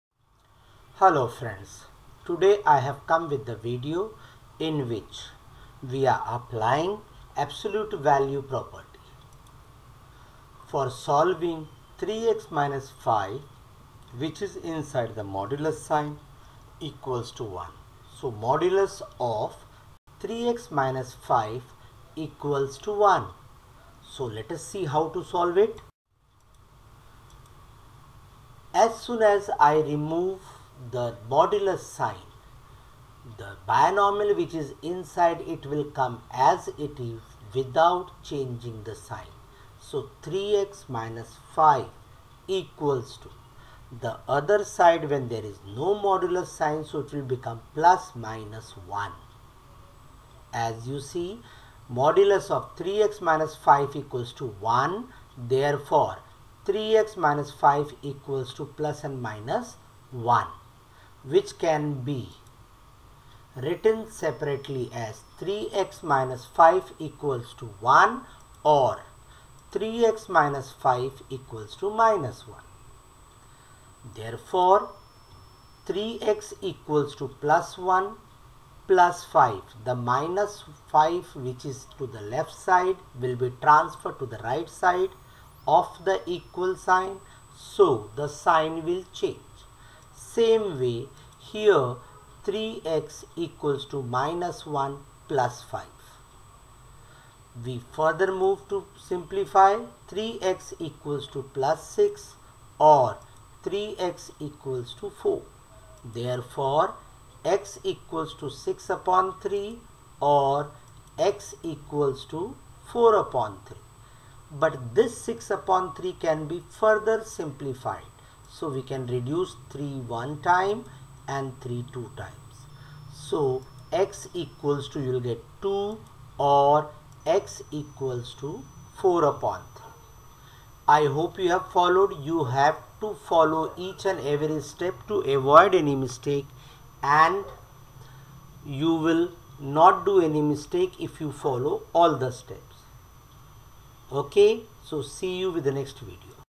Solution of |3x-5|=1 using Absolute value property: part - 1
This video is created by Yogesh Sharma Sir which help u to solve the given sum with the help of absolute value of a real number